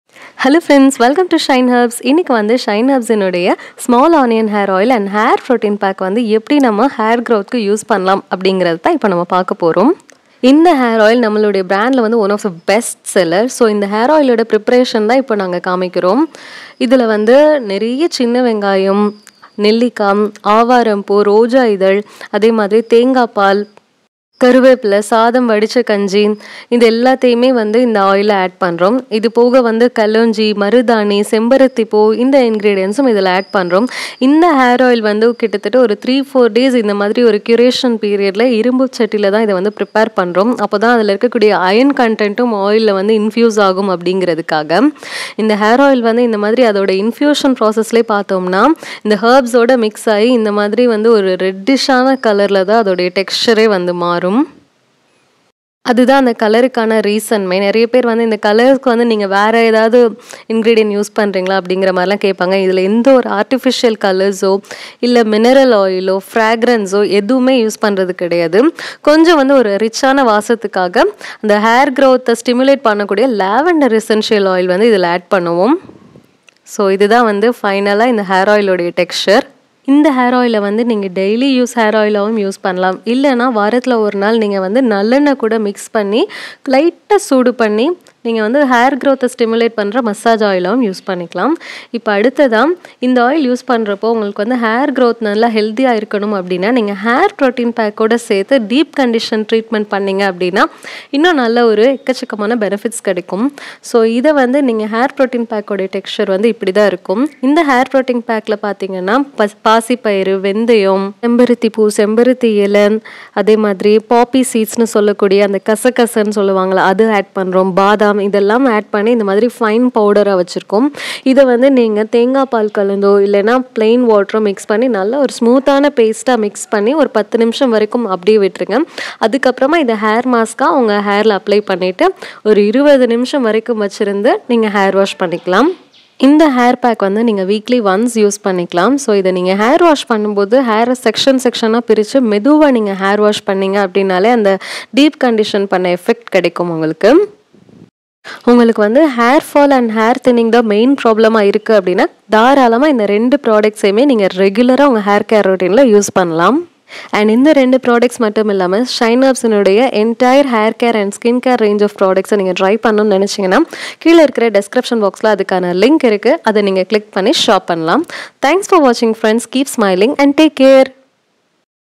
How to get healthy hair with Shine Herbs Small Onion Hair Oil & Hair Protein Pack?
Hi Shine Tribe!

Welcome back!! 🌻